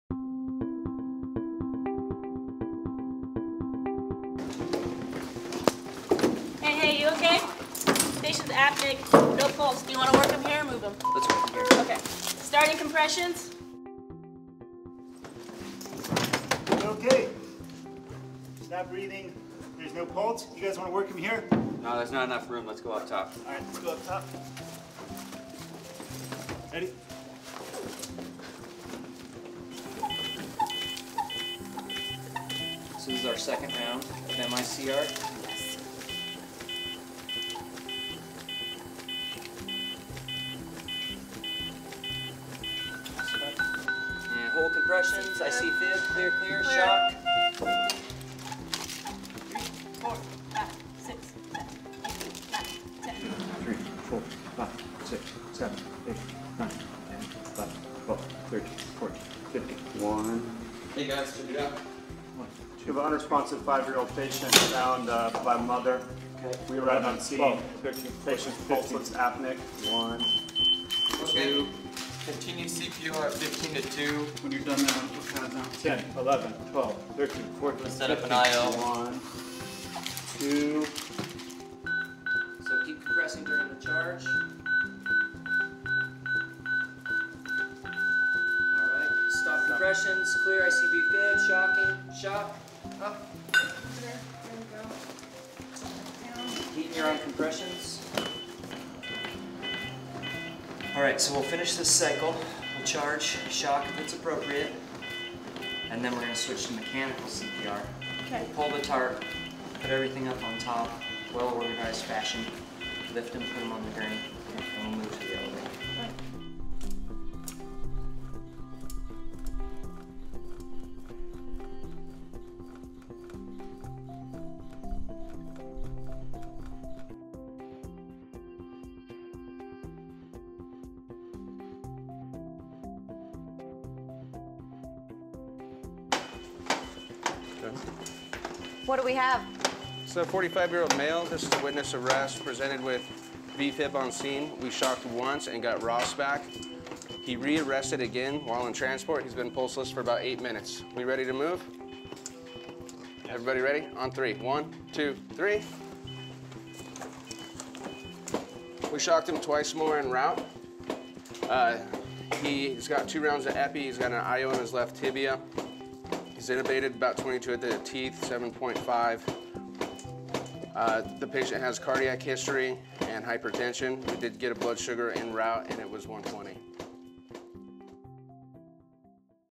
Challenges in High Performance CPR
Common problem areas and how to avoid them in your resuscitations.